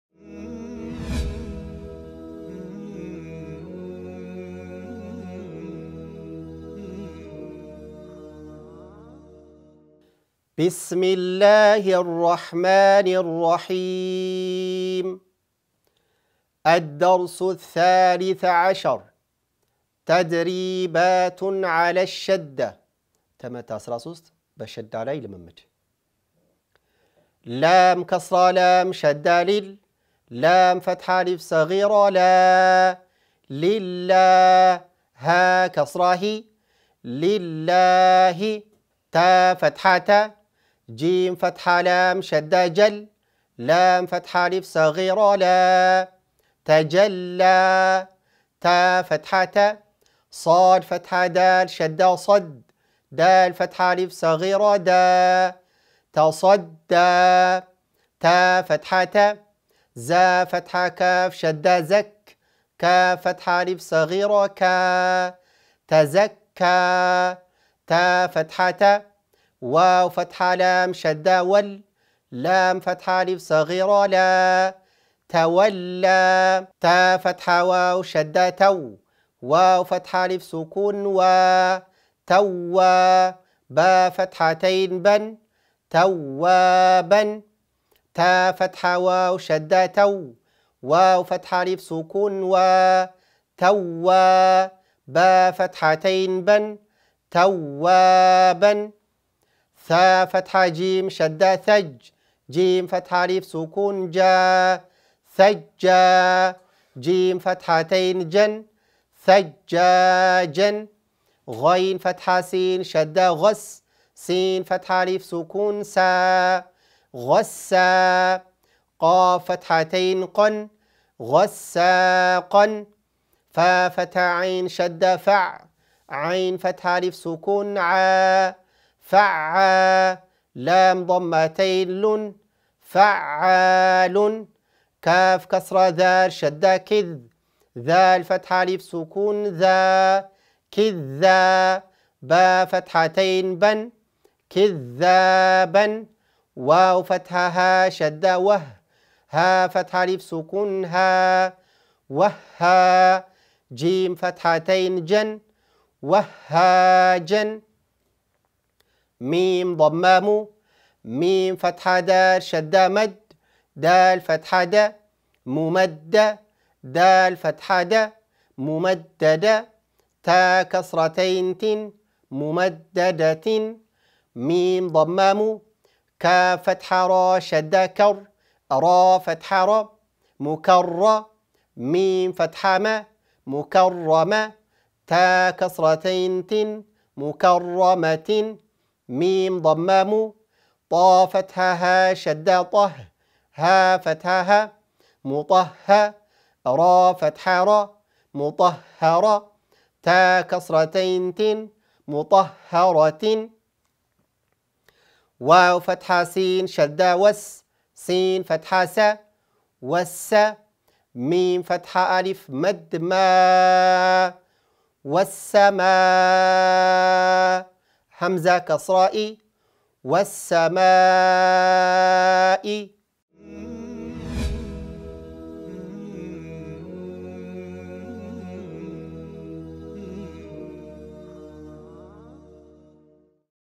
ክፍል 50 ገፅ 68 ደርሱ ሣሊሠ ዓሸር / ትምህርት አስራ ሶስት || በሸዳህ ላይ ልምምድ || የአል ቃዒደቱን ኑራኒያን ንባብ || ለጀማሪዎች
⏭አድስ መፅሐፍ ለጀማሪዎች የመማሪያ መፅሐፍ
📚 የአል_ቃዒደቱን ኑራኒያህ ንባብ እና የተጅዊድ ህጎች
📍ሙሉውን በጥሞና ከተከታተሉ በኋላ ወደ ቀጣዩ ቪድዮ ይሸጋገሩ

በሚከተሉት ማህበራዊ ትስስር ገፆች ያገኙናል ይከተሉን